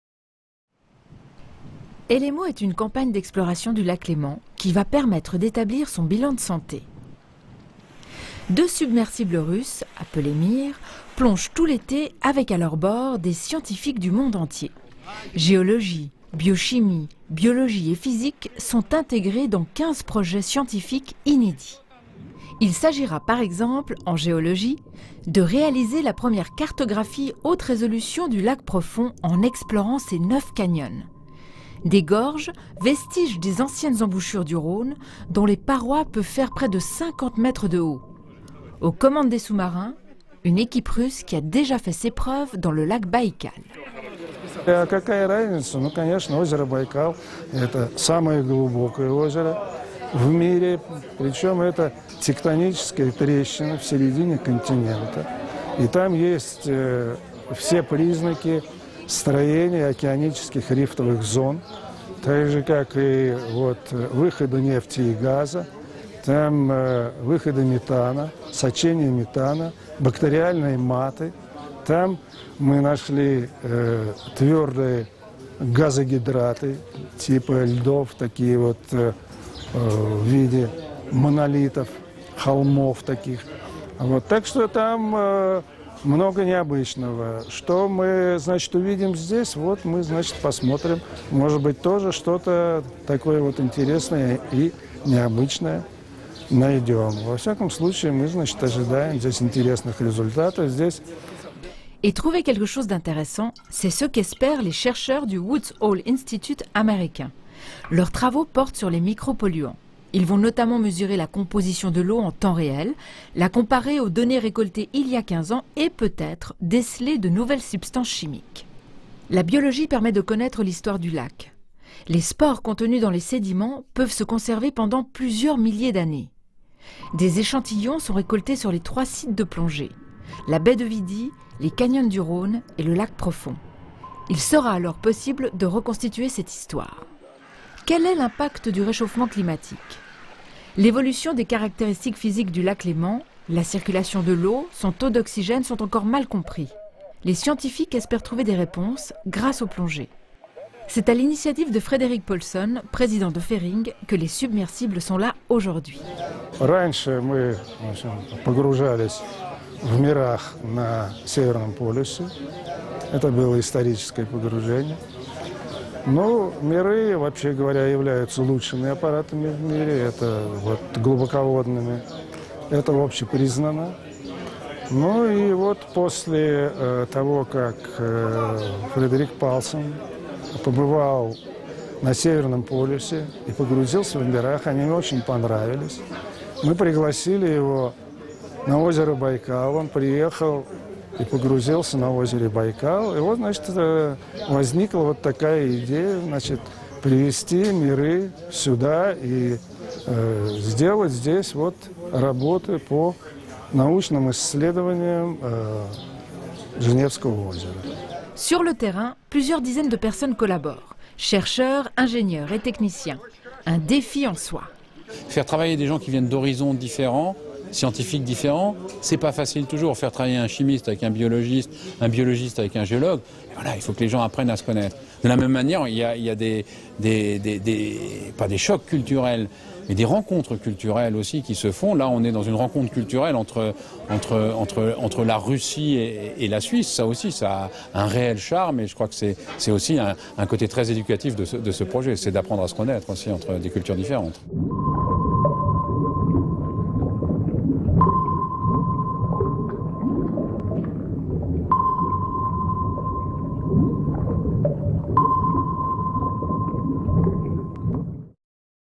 Projet ELEMO 2011
Etablir un bilan de santé du lac Léman avec les sous-marins russes.
20 juillet 2011.
EPFL.